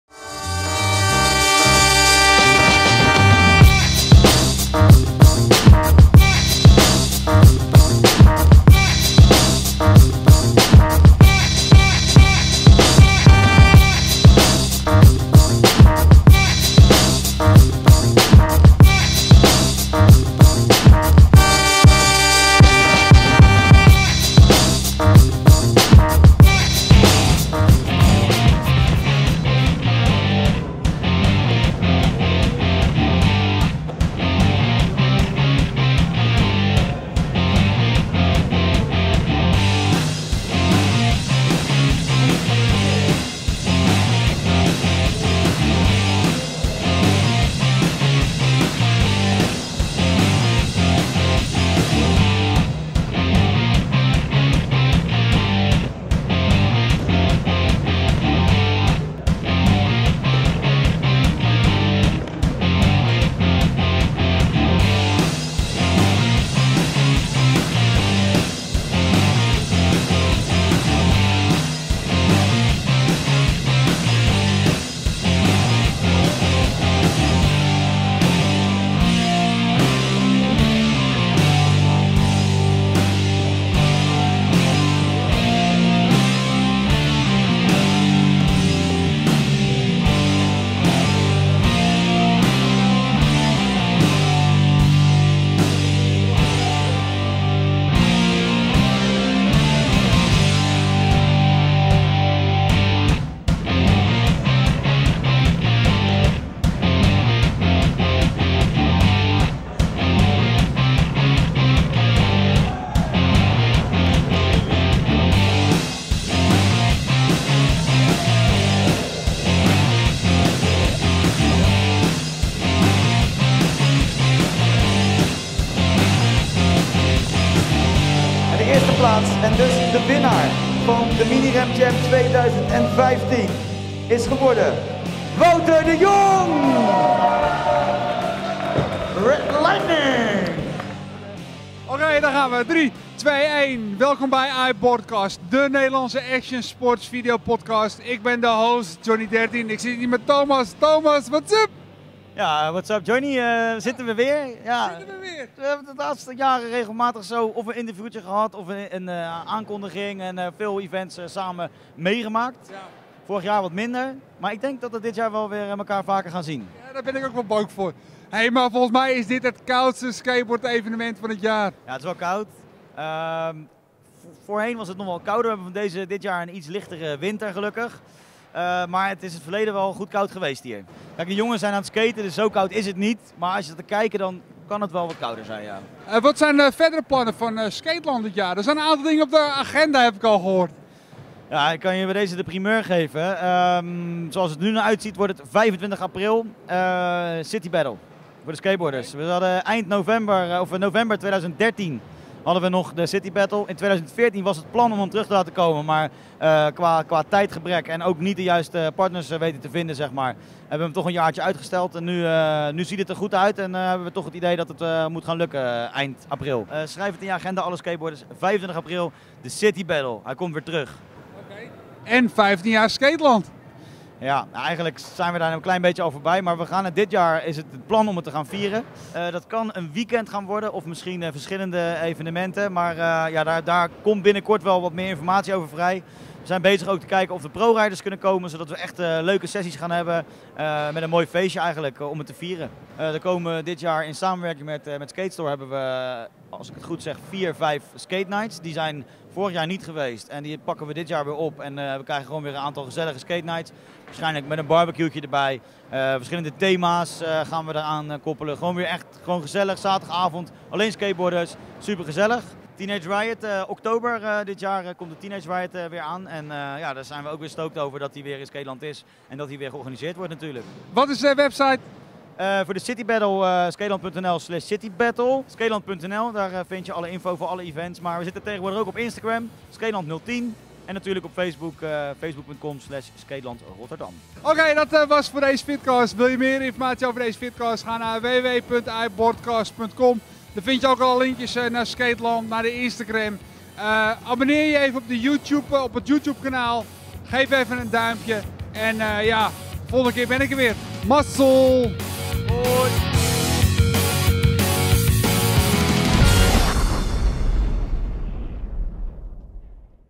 Skateland Mini Ramp Jam - IBCT336
Eind januari. Buiten valt er sneeuw dus is het tijd voor de koudste skateboard wedstrijd van het jaar. De Skateland Mini Ramp Jam. Een nieuwe generatie kids stond klaar om het tegen de oude generatie op te nemen. Gaat het hun lukken om te winnen?
Na de prijsuitreiking hadden wij een interview met Thomas van Bemmelen, host van Skateland. Wat gaan jullie doen in 2015?

Nieuwe intro song door The Passion HiFi @soundcloud.com

Shownotes:
00:00 -- New Intro
00:10 -- Sponsors Vans Off the Wall - Burnside Skatepark
00:25 -- Mini Ramp Jam
02:10 -- Prijsuitreiking beste 3 skateboarders en de beste crash.
02:22 -- Intro Johnny 13 & Thomas van Bemmelen Koudste wedstrijd van het Jaar?
03:05 -- Wat voor skateboard events gaan jullie in 2015 organiseren?
03:15 -- City Battle 25 april 2015
03:42 -- 15 jaar Skateland Rotterdam
04:02 -- Skatenight presented by Skatestore.nl
04:22 -- Teenage Riot
08:35 -- De website
08:50 -- Closer